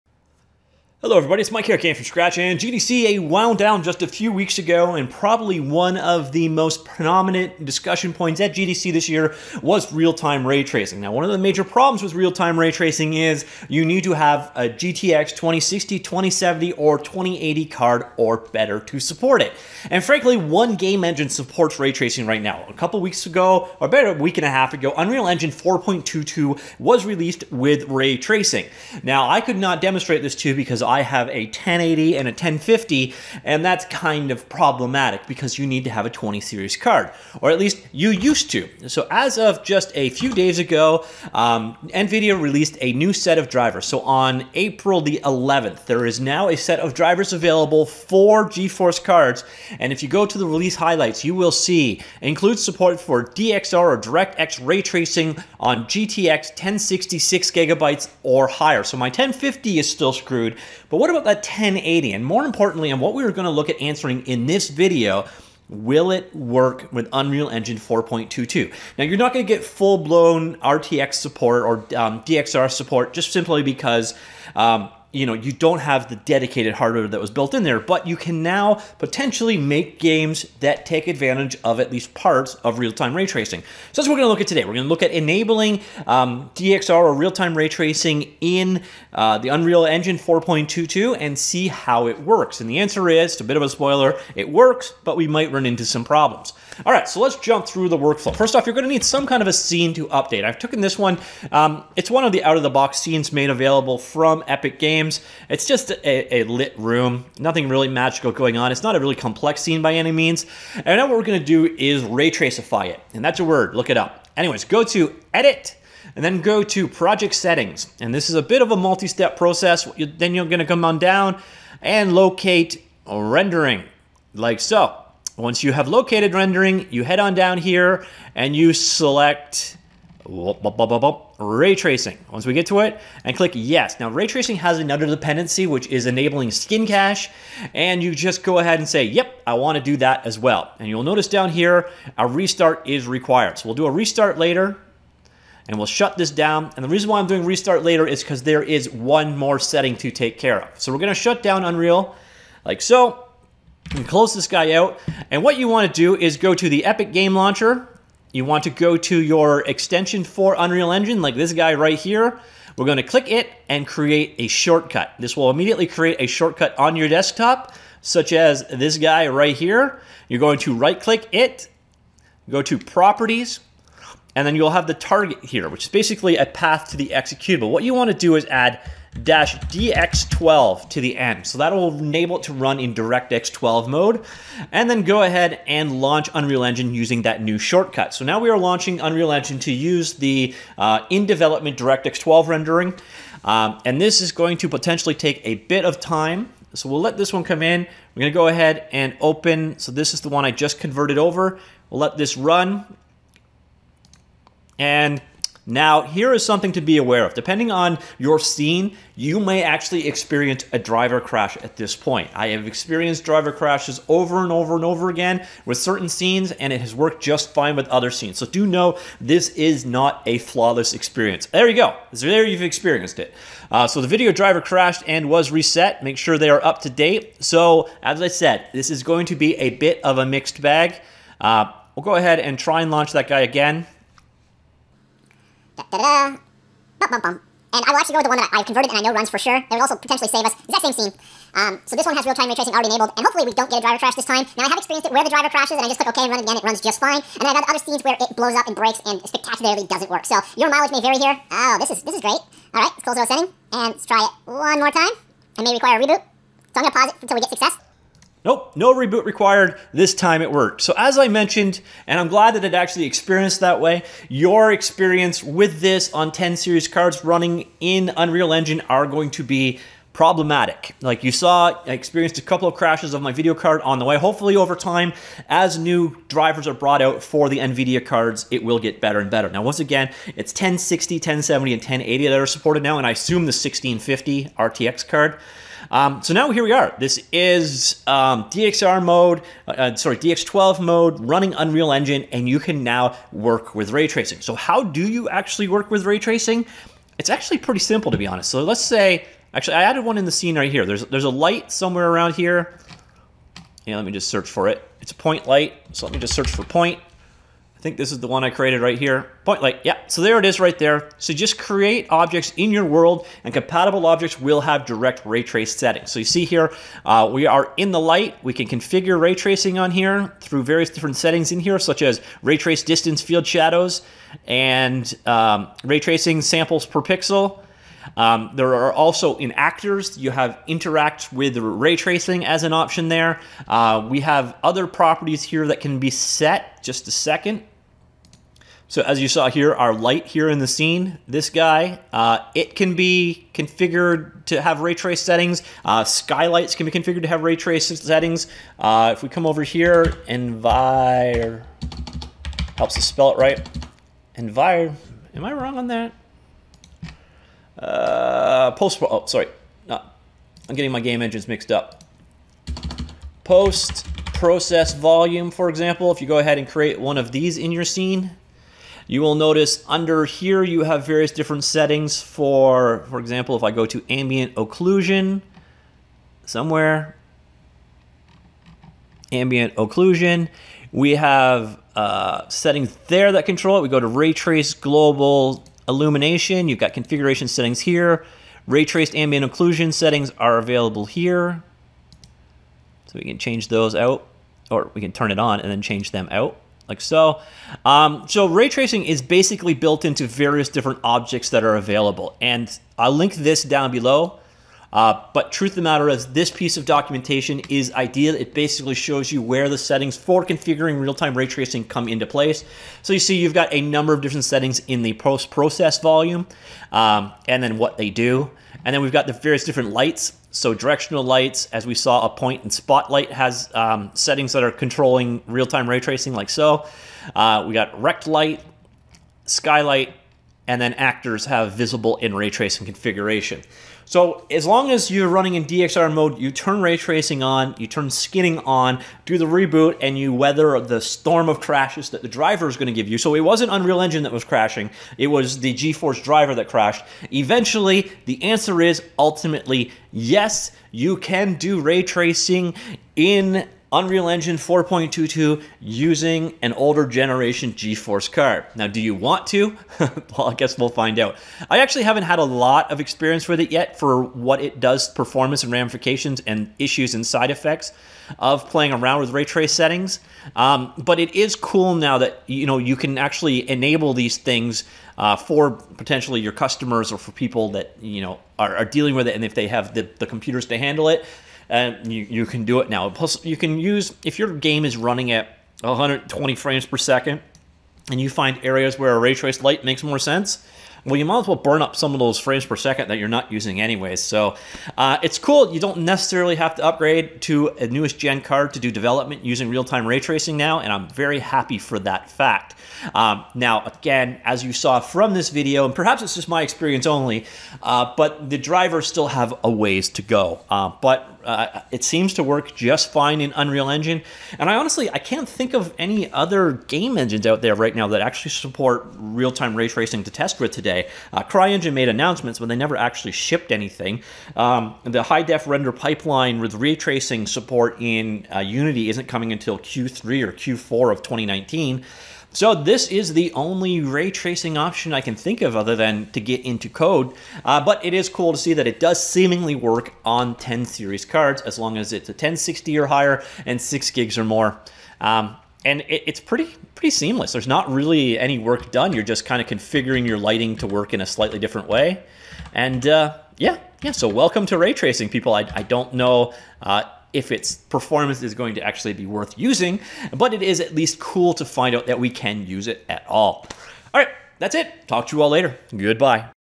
Unreal Engine Real-time Raytracing using Older GPUs?
So can you create DXR or realtime raytracing games using Unreal Engine 4.22 now that new Nvidia drivers has shipped supporting DXR on more devices?  As you will see in this video...

sorta.  It sorta works.